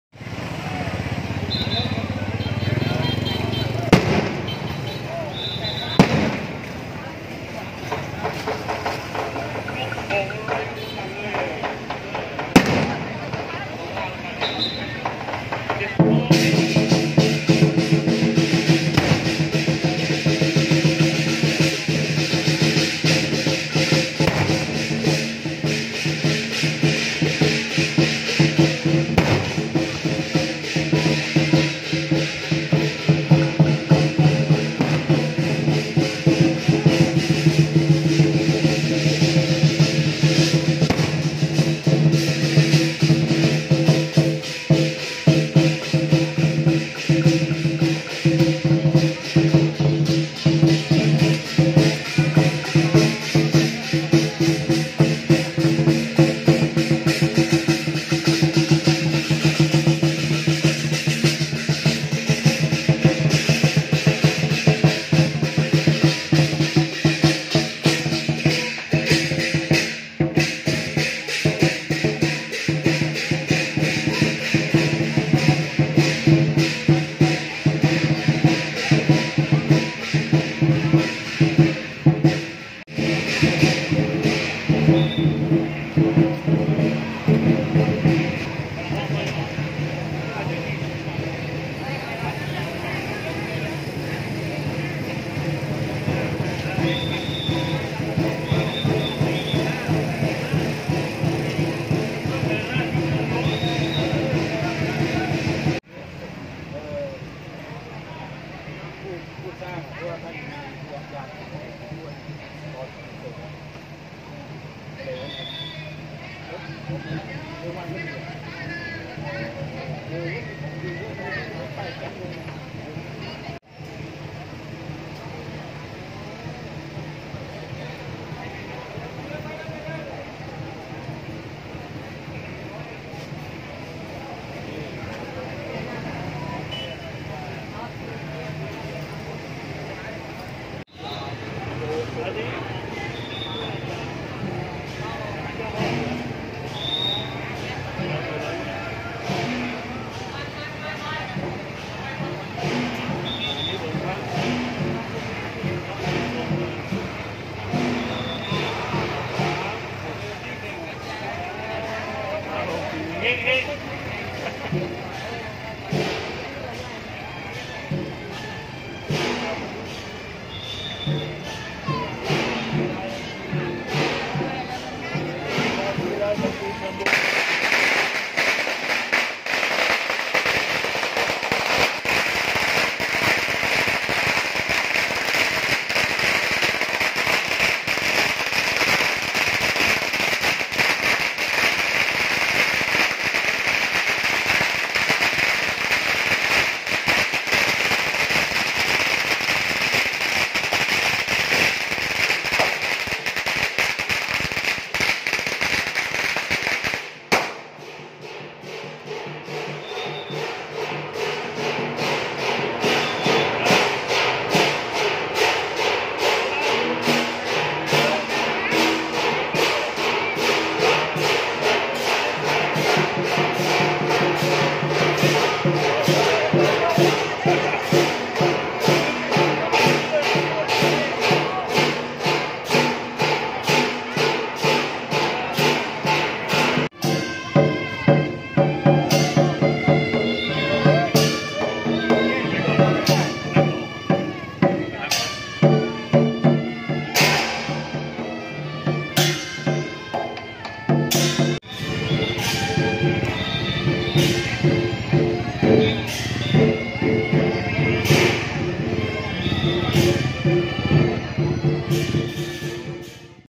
ประเพณีแห่พระสะเดาะเคราะห์ ประจำปี ๒๕๖๔
มูลนิธิมิตรภาพสามัคคี (ท่งเซียเซี่ยงตึ๊ง) หาดใหญ่ จัดงานประเพณีแห่พระสะเดาะเคราะห์ประจำปี 2564 ระหว่างวันที่11-20 ก.พ. 2564

โดยวันที่19 ก.พ. 2564 เวลา 08.30 น. อัญเชิญองค์หลวงปู่ไต้ฮงโจวซือและองค์เทพเทวาทุกองค์ภายในศาลเจ้า ประทับบนรถยนต์ออกโปรดลูกหลานไปตามถนนต่าง ๆ ภายในเมืองหาดใหญ่ เพื่อเป็นสิริมงคลแก่สาธุชน ร้านค้า ห้างร้านต่างๆ